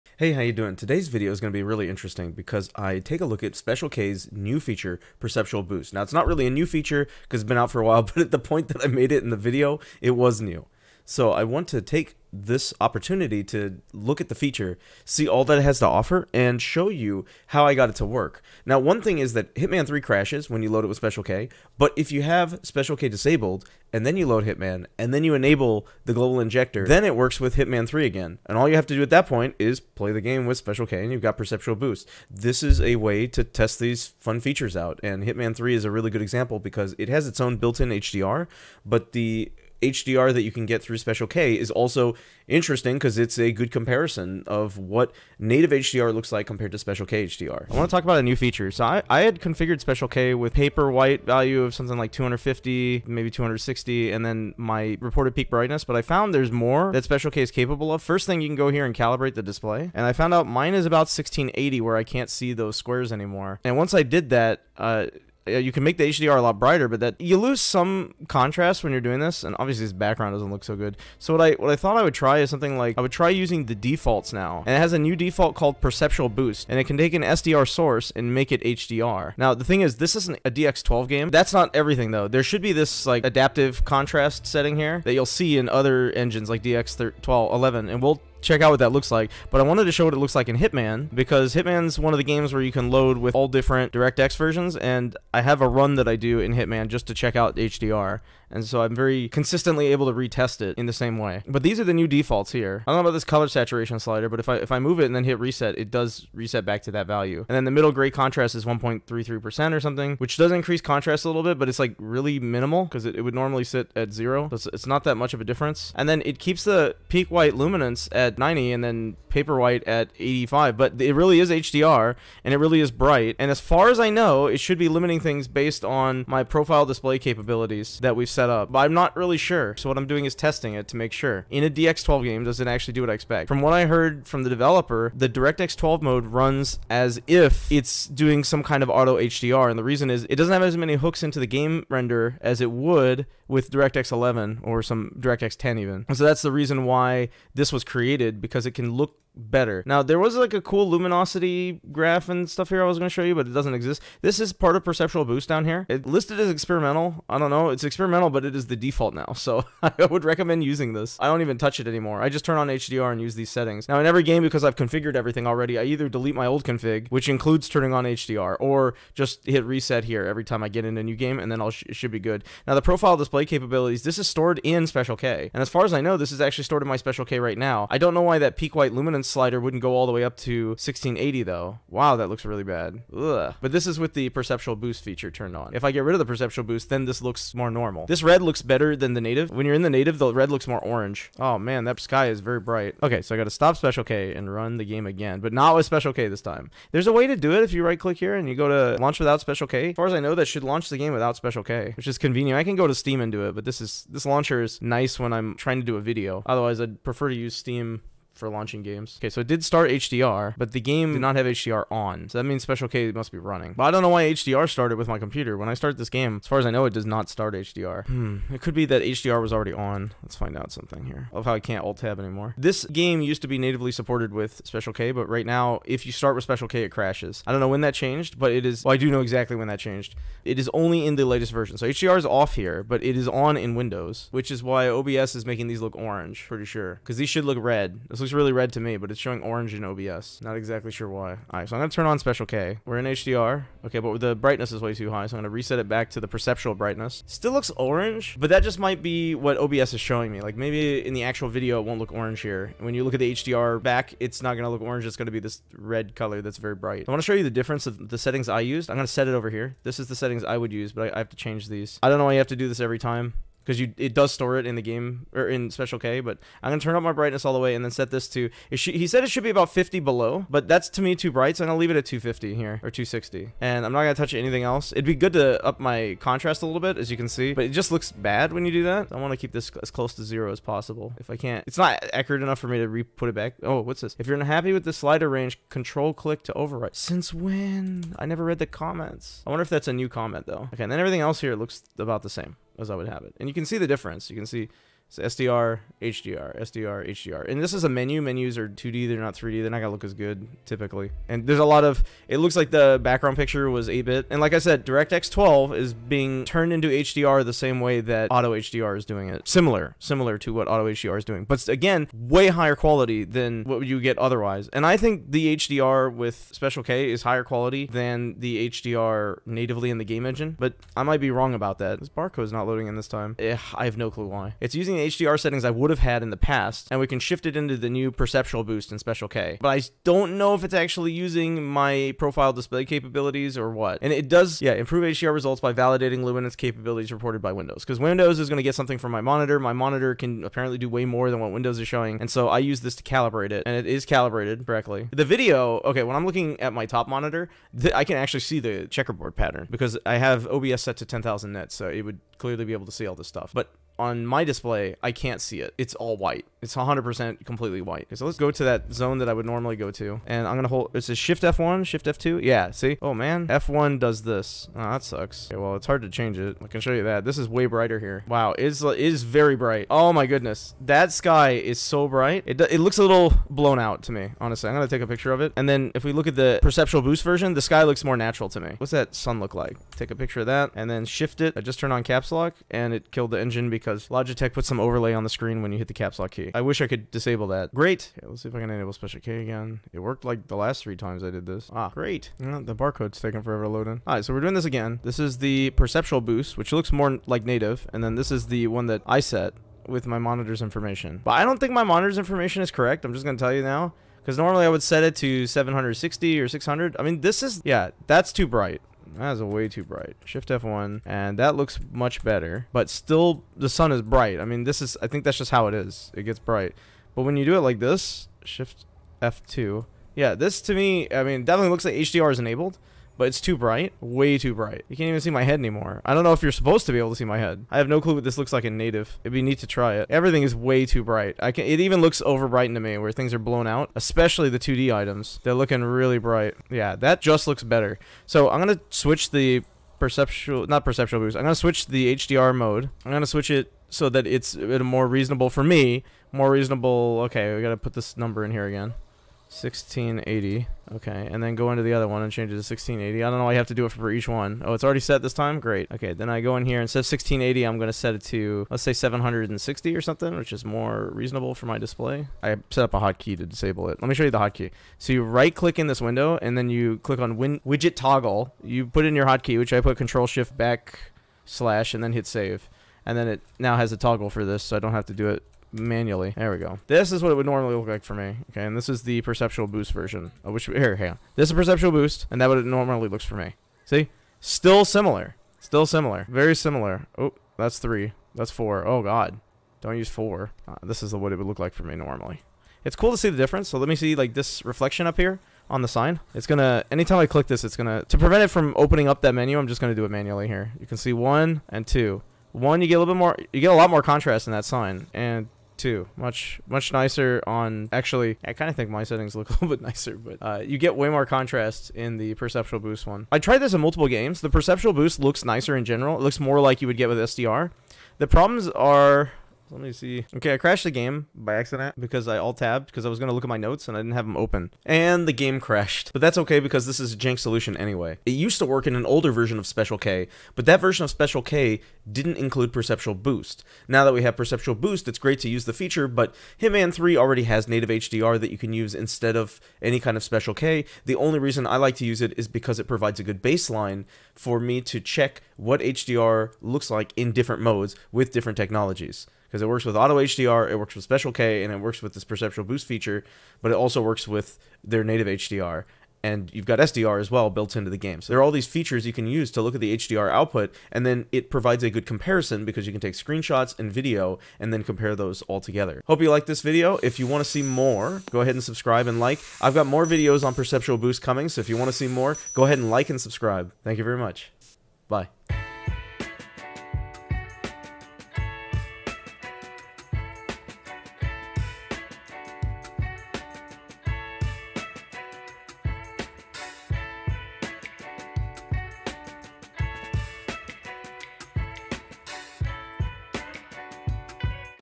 BREAKTHROUGH in game HDR: Perceptual Boost
Special K just got a brand new feature: Perceptual Boost. This feature lets you render SDR as HDR retaining a ton of the original contrast with the check of a button. It's much easier than fiddling with the manual controls, and it consistently looks good!

This is a major breakthrough in HDR tech. A ridiculous number of games, even brand-new ones, lack HDR. You can add HDR to them using Special K, and the new Perceptual Boost feature makes it so easy now!